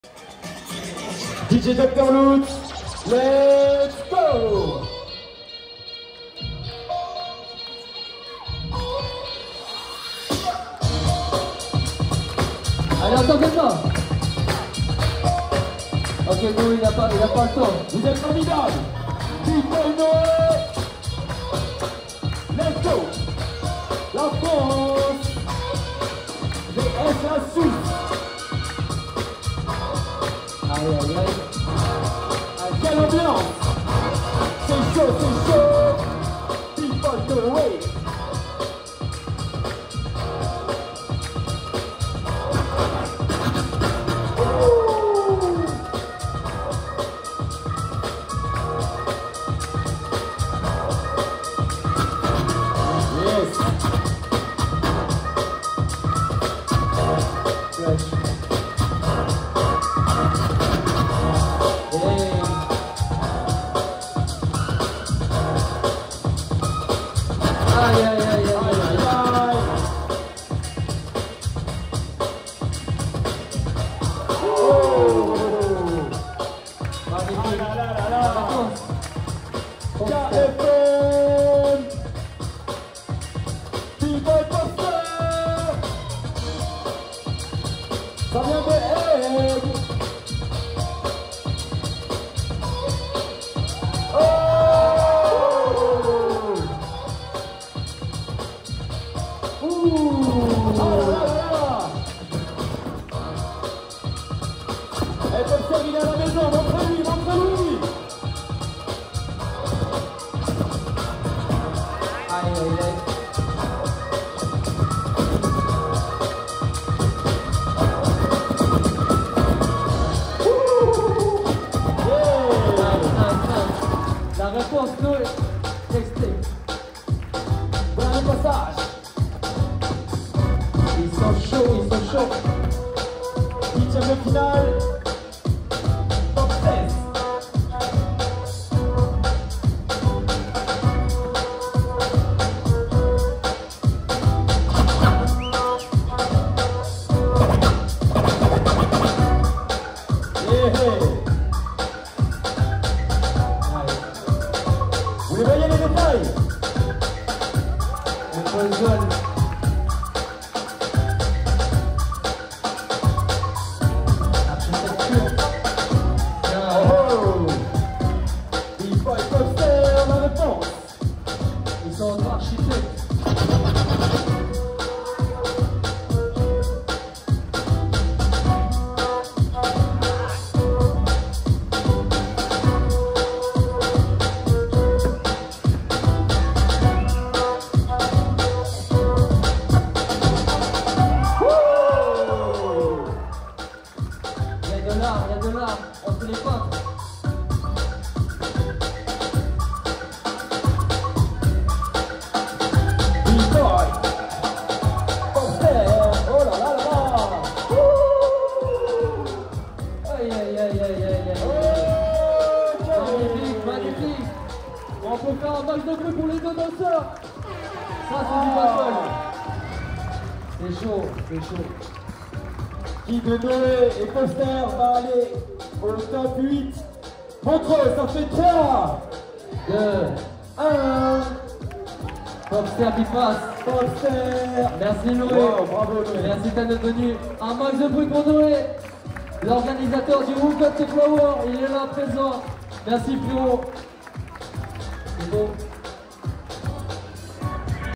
Noé France vs Popster Switzerland - TOP 16 World Final BREAKQUALITY 2024 - MONTREUX FORUM
WINNER POPSTER